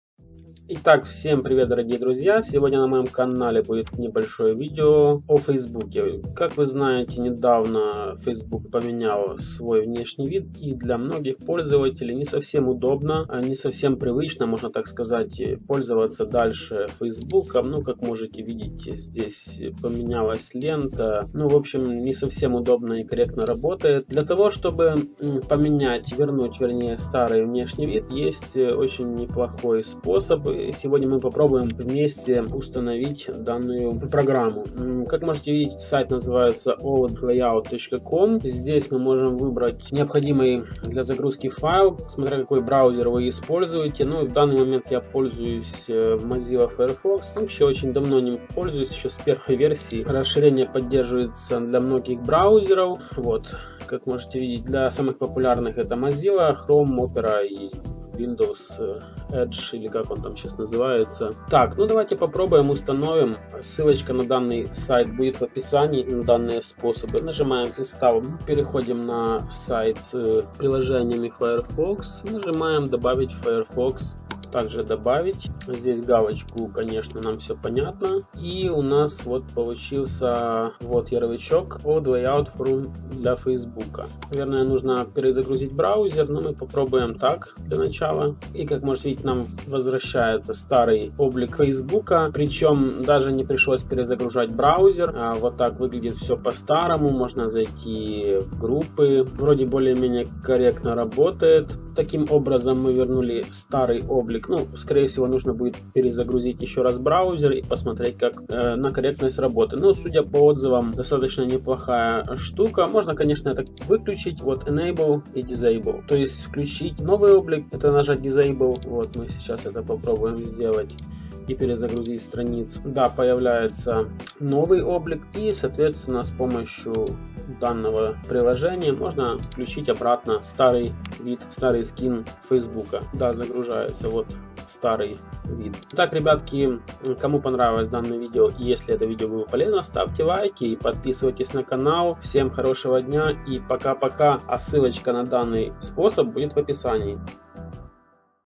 Как вернуть старую версию фейсбука. Old Layout for Facebook
Приложение для тех кто хочет вернуть старую версию Facebook. Работает во всех популярных браузерах.